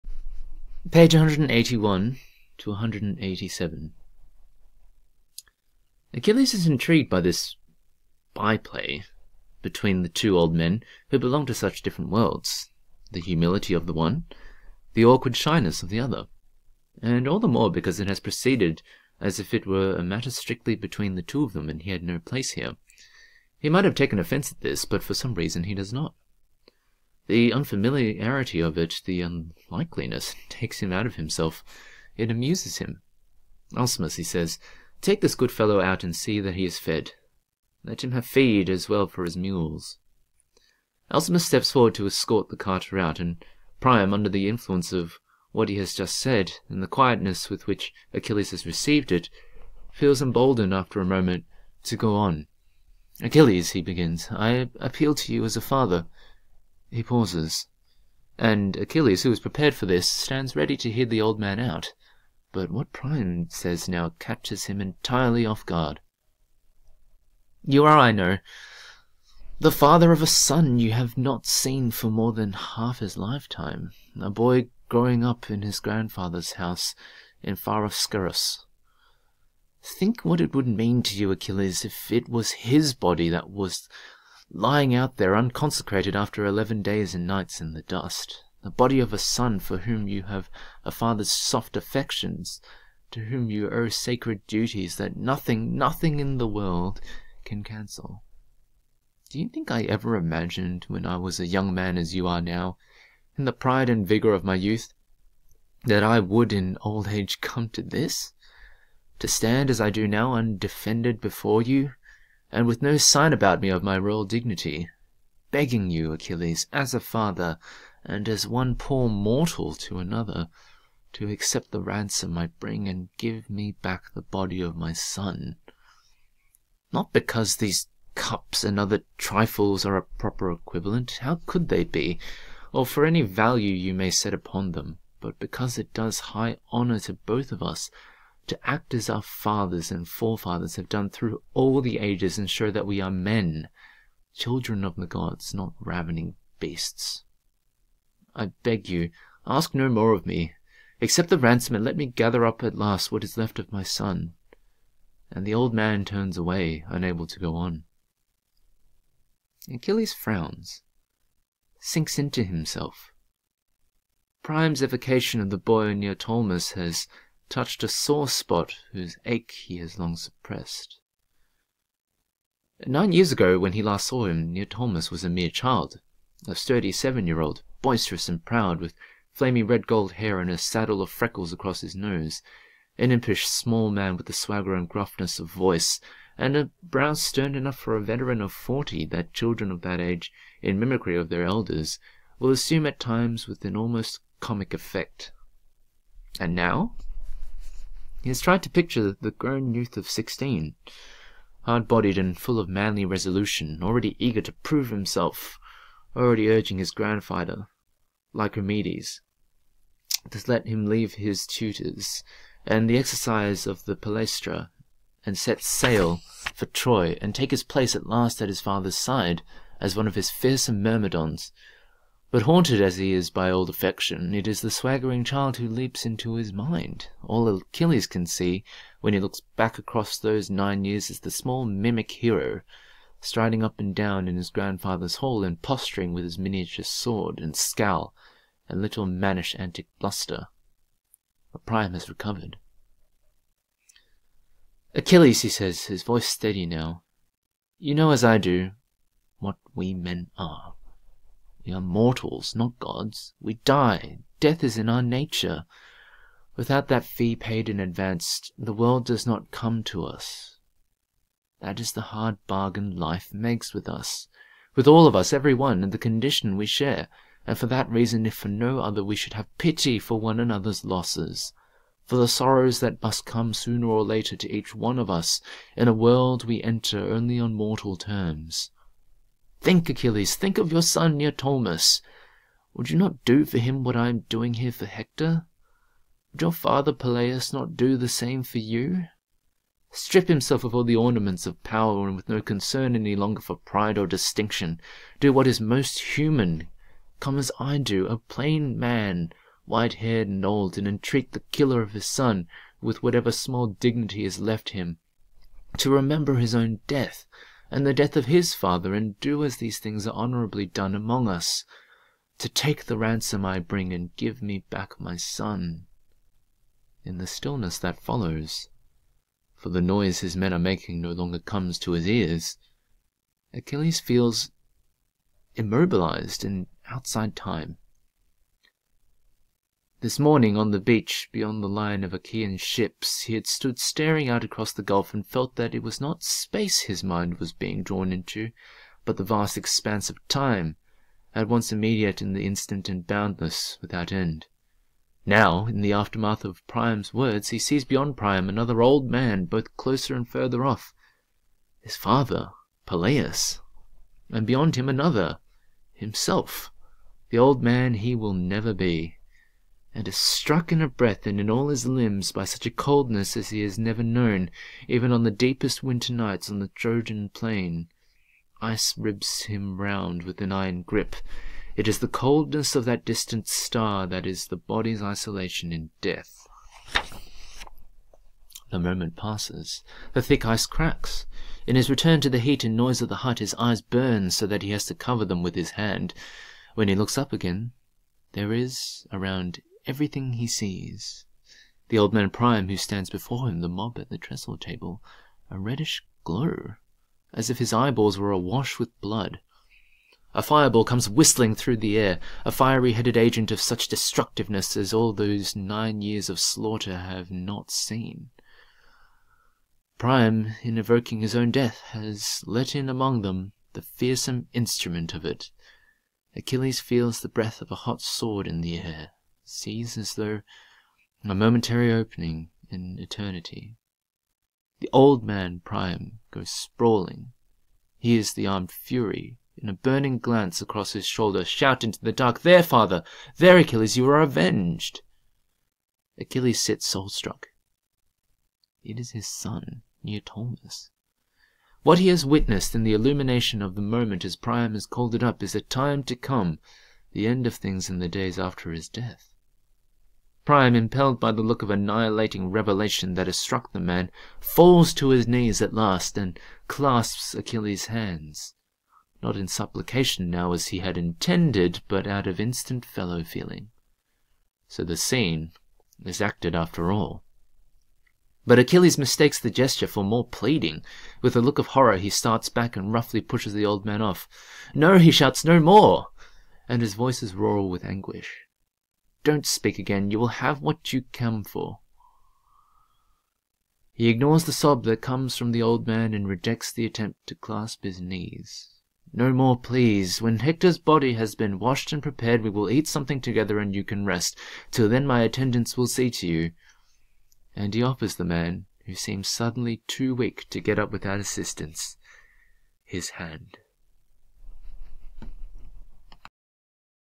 Ransom by David Malouf - Chapter 4: Priam appeals to Achilles; Achilles sees a vision of the future
pp. 181-187